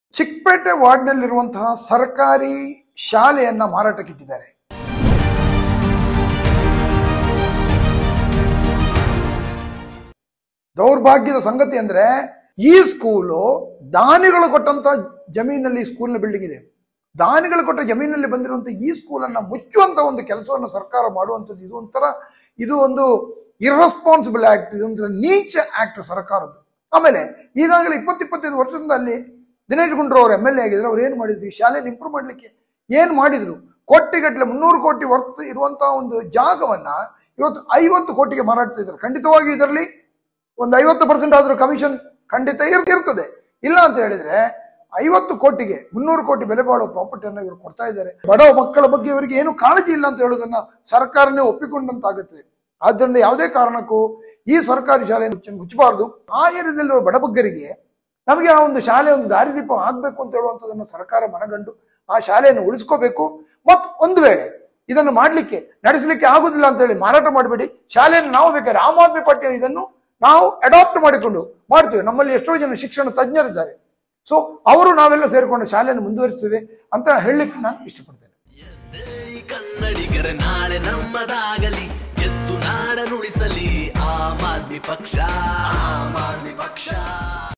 300 ಕೋಟಿ ಬೆಲೆ ಬಾಳೋ, ದಾನಿಗಳು ಕೊಟ್ಟ, ಸರ್ಕಾರಿ ಶಾಲೆ ಜಾಗವನ್ನ, 50 ಕೋಟಿಗೆ ಮಾರೋಕೆ ಹೊರಟಿರುವ ಪರಮ ನೀಚರು ಯಾರು?
If you can't handle let, we will adopt and show you how it's done! AAP Challenge!